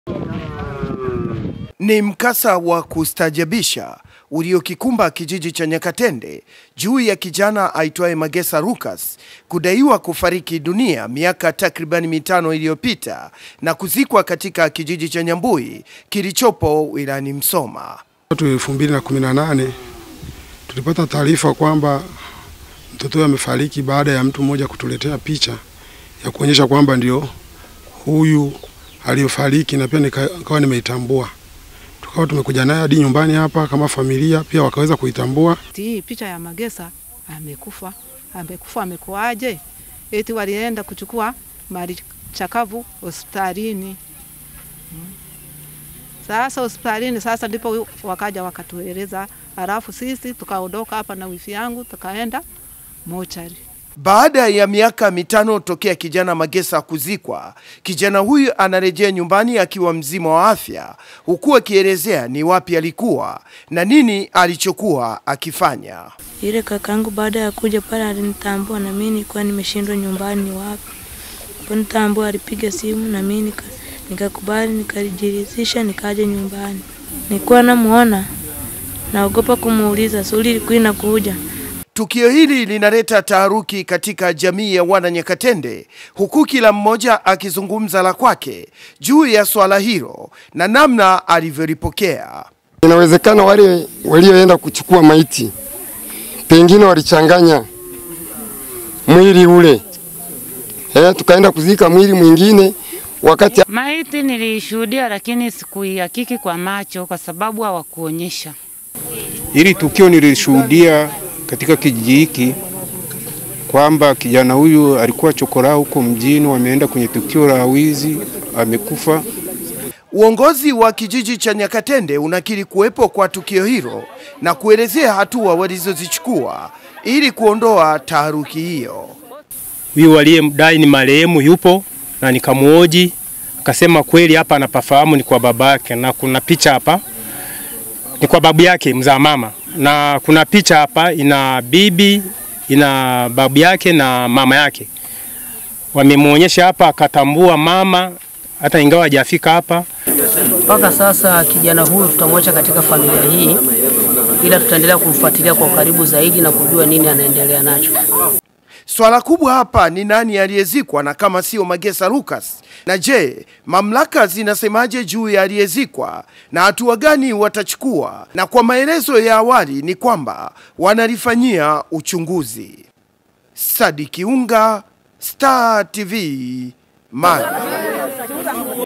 ALIYEFARIKI MIAKA MITANO ILIYOPITA AONEKANA AKIWA HAI/AZUA TAHARUKI KWA FAMILIA NA WAKAZI WA MUSOMA
Wakazi wa kijiji cha Nyakatende halmashauri ya wilaya ya Musoma mkoani Mara wameingiwa na taharuki kufuatia kijana aitwaye Magesa Lucas kulejea nyumbani baada ya miaka sita tokea kufariki dunia na kuzikwa kijijini kwao Nyambui.